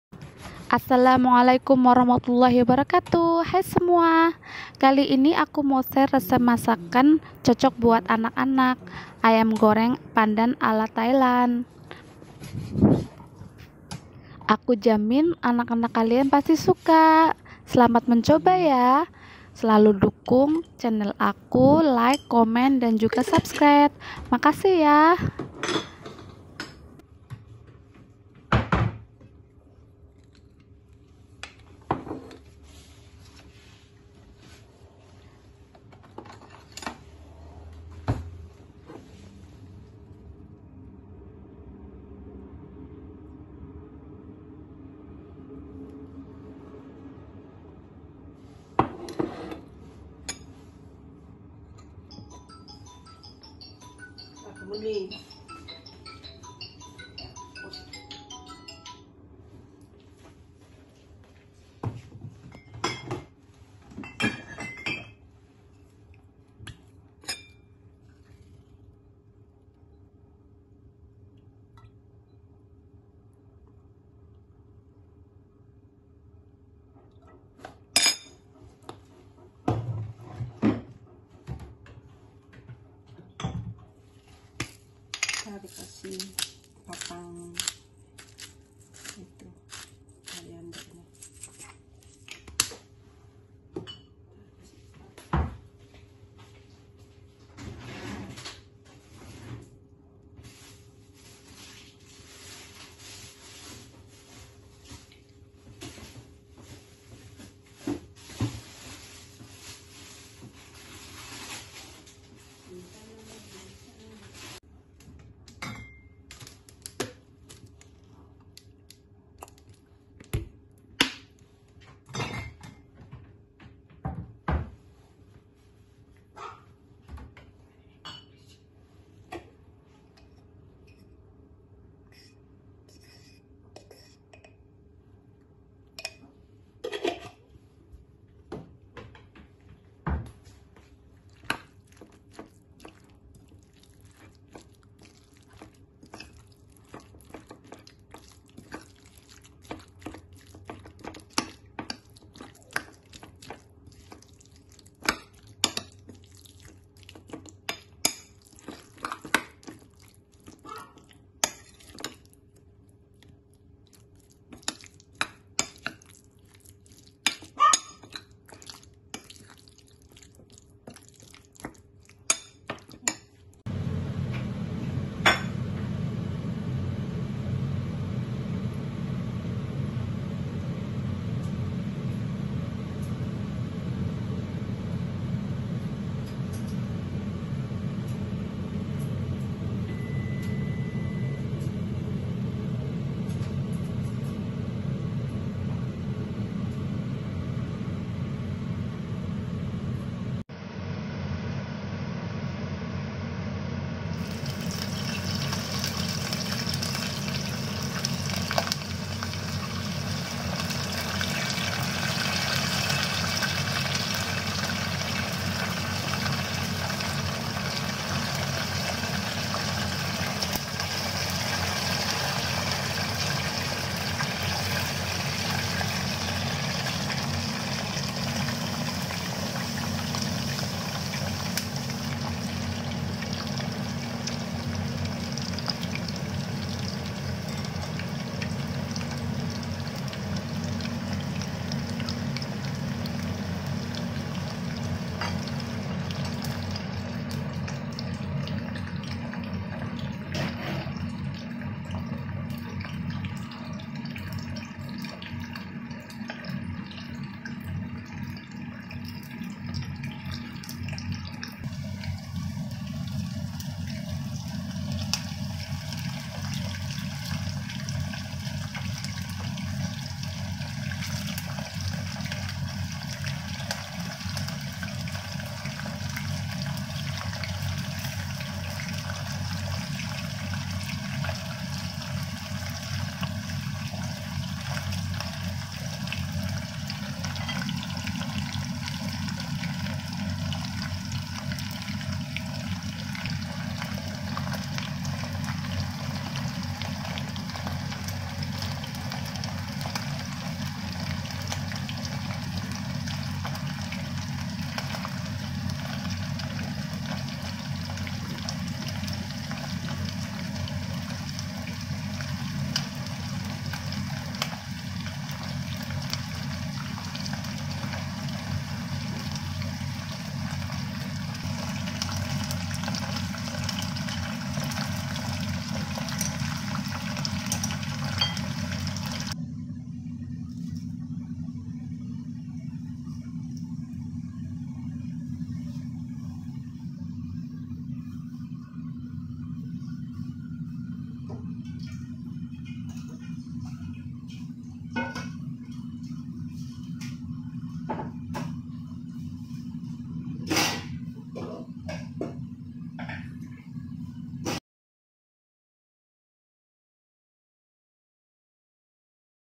ayam goreng daun pandan ala Thailand
bahan-bahan:
ayam tanpa tulang
daun pandan
bawang putih
jahe
daun jeruk

bumbu
saus tiram
fish source Thailand (bisa diganti merk lain)
tepung maizena
minyak

caram memasak:
1.cincang bawang putih,jahe,daun jeruk
2.potong ayam kasih cincangan bawang putih, jahe, daun jeruk saus tiram, fish sous, tepung maizena,minyak, campur uleni
3.taruh d kulkas
4.bungkus pakai daun pandan
5.goreng
6.sajikan